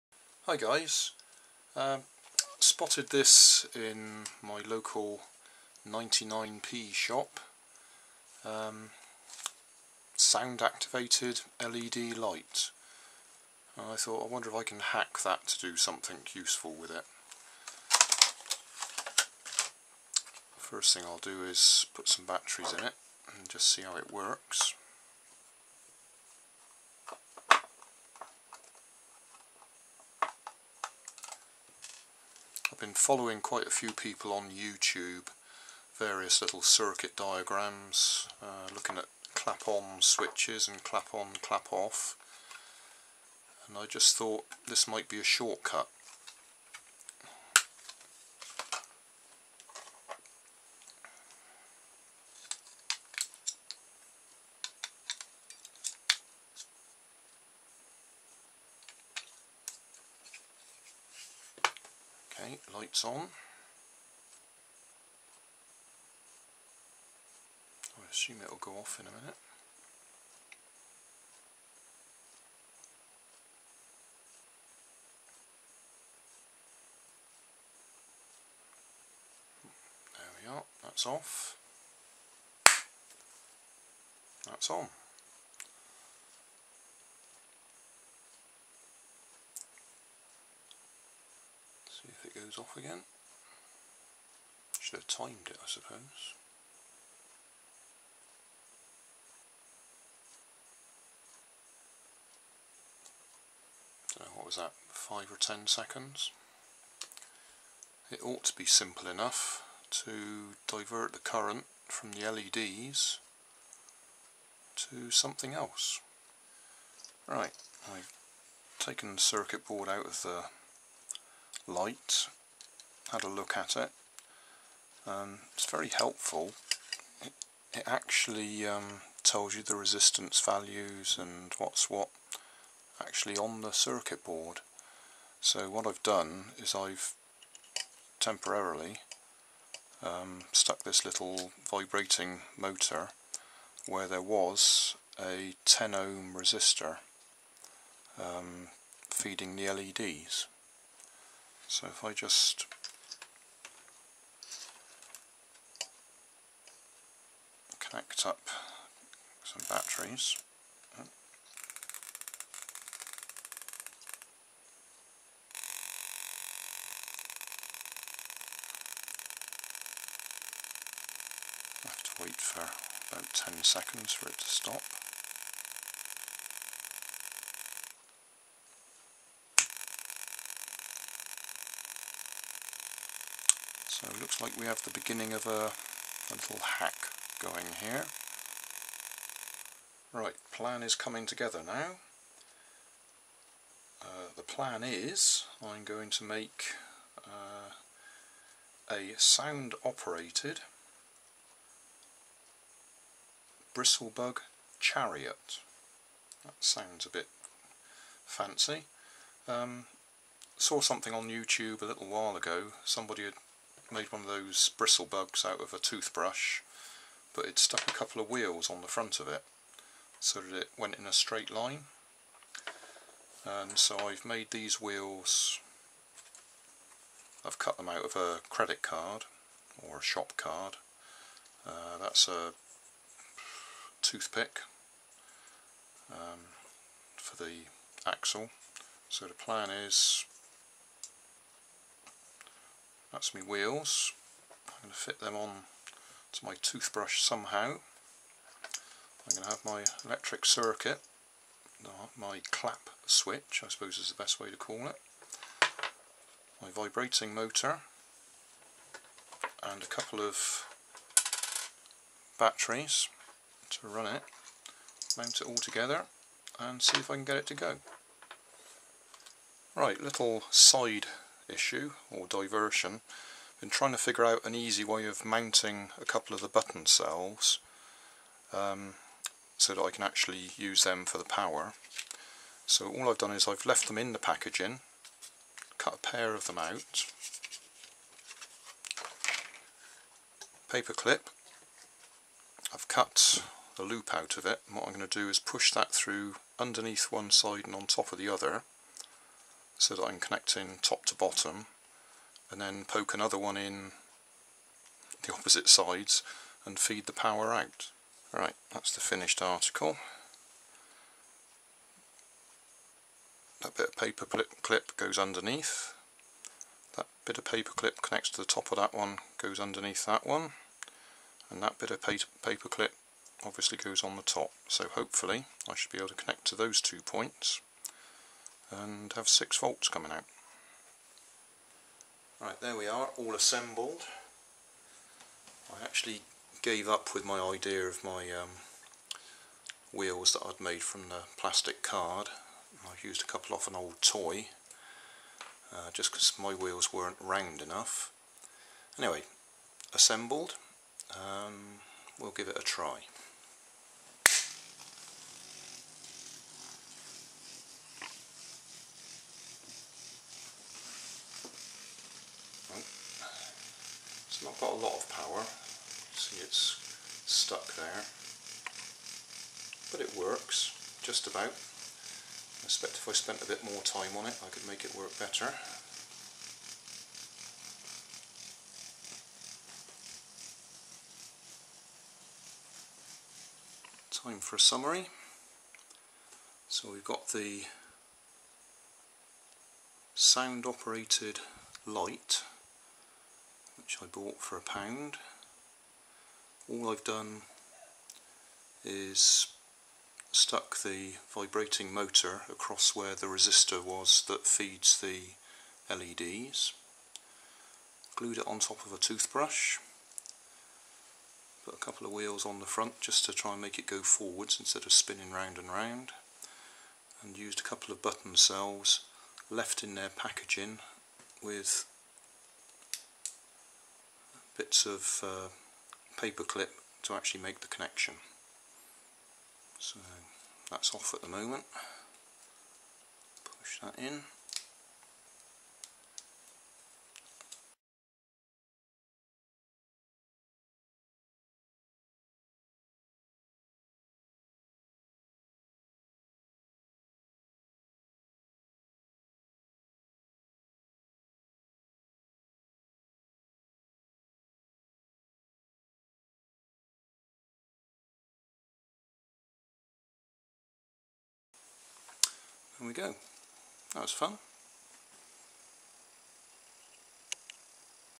Let's Hack a Sound Activated LED Light
I have been watching videos on YouTube on making Clap On and Clap On/Clap Off switches with the intention of making one to activate a toy. Then I spotted this Sound Activated LED Light in my local 99p Store and thought it might be worth trying it as a shortcut in making one of those circuits.

I have simply replaced the resistor that feeds the LEDs with one of my vibrating motors taken from a vibrating toothbrush. Probably not the best way to do this, but it it quick and easy.
I haven't spent time fine tuning the end result so the balance is not quite right and I may not have used the best toothbrush or wheels for the job but it does work and you can see the result. I didn't remove any of the LEDs either so that is another tweak I could try.
The bristle bug chariot I finally made was inspired by a video by 'Attila Blade' titled "Solar bristlebot car" so here is a link to his work.